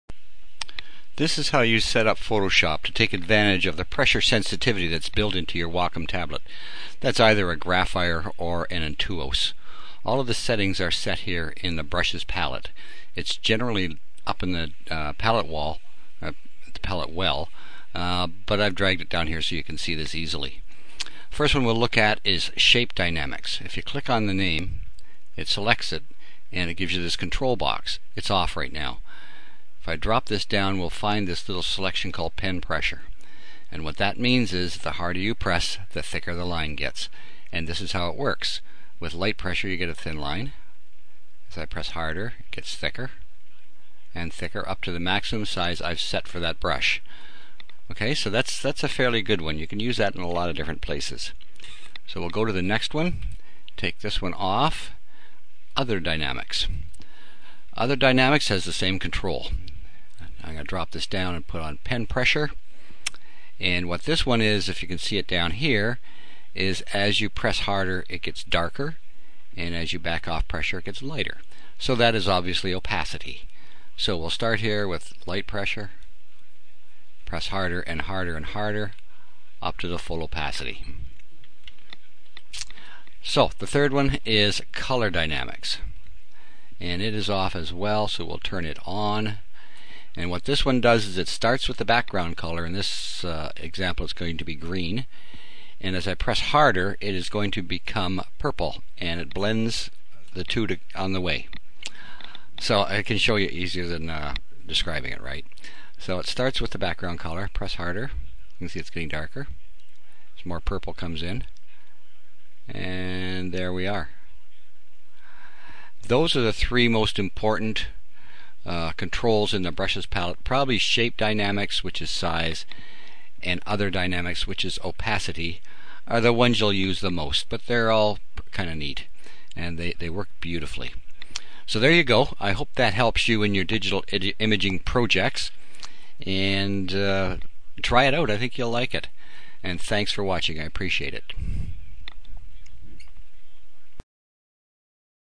Setting Photoshop Brushes for a Wacom Tablet
How to set the brushes in Photoshop to take full advantage of the pressure sensitivity of a Wacom Tablet (Graphire,  Intuos and the new Bamboo).